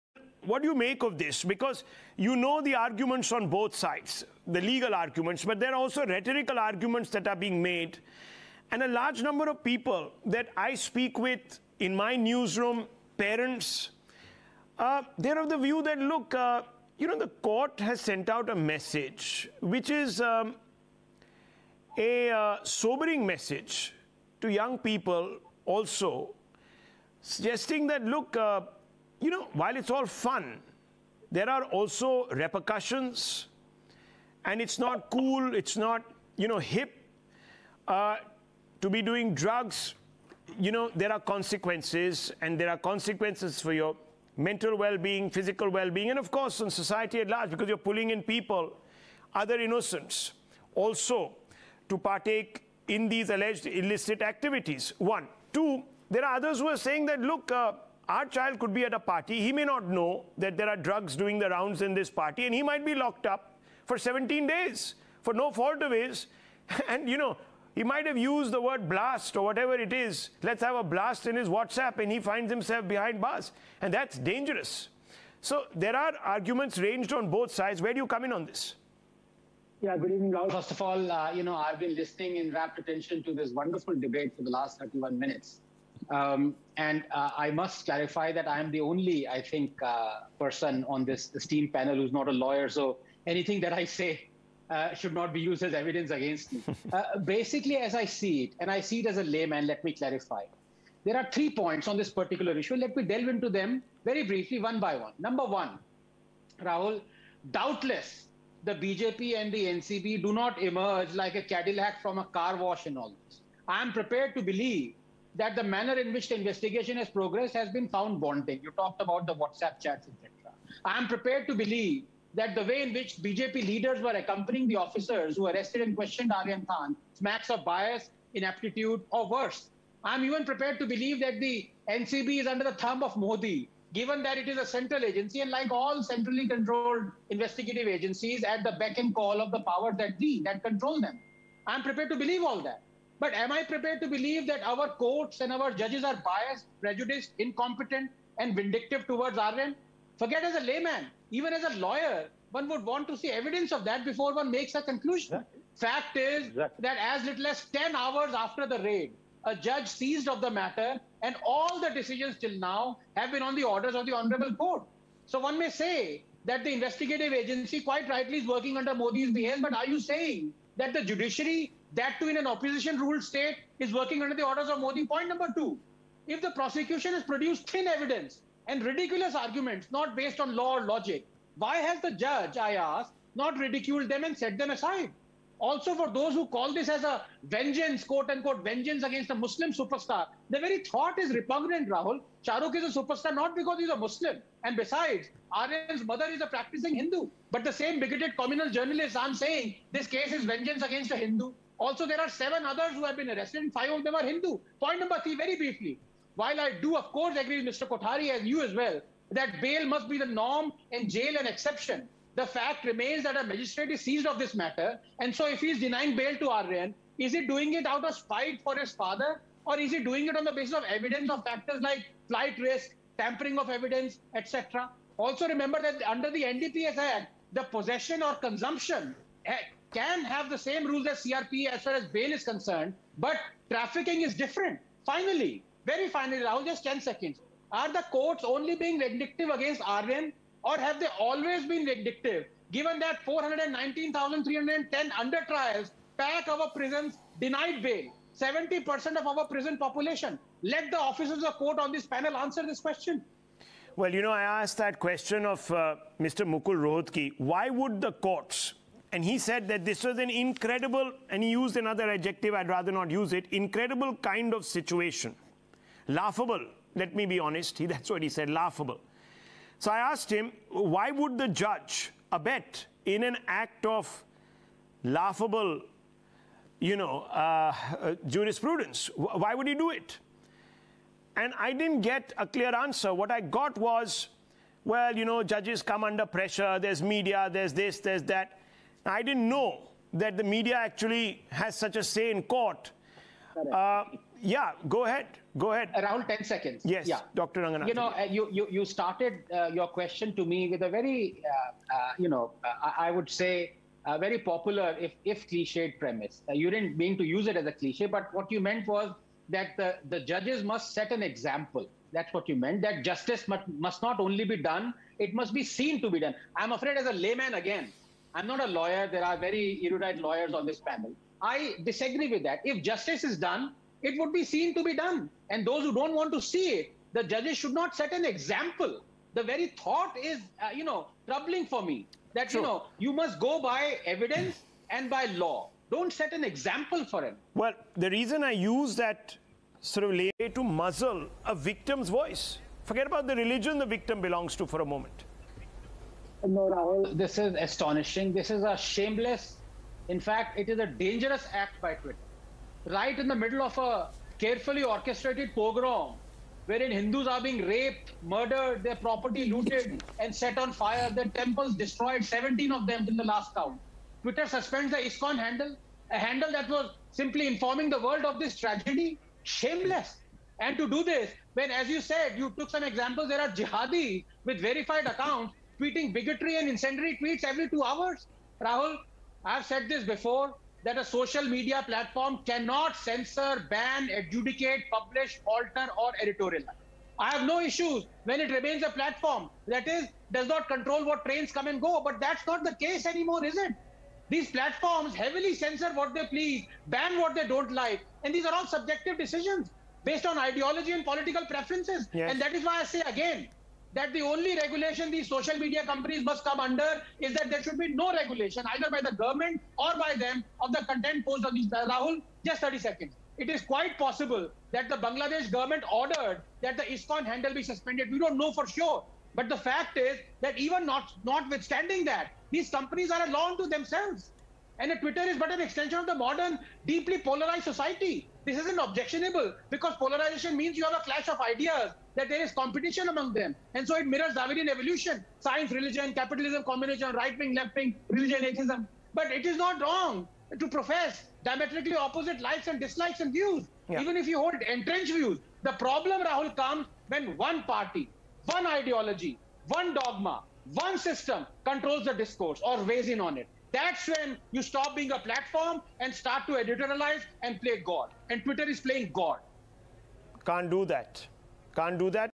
Intellectual Indian - Anand Ranganathan Talk About Drug Case & Twitter Bias on Hindu Attacked in BD
Anand Ranganathan latest debate
Twitter bias on Hindu attacked in Bangladesh
Hindu
Intellectual Indian
Twitter Bias
Drug Case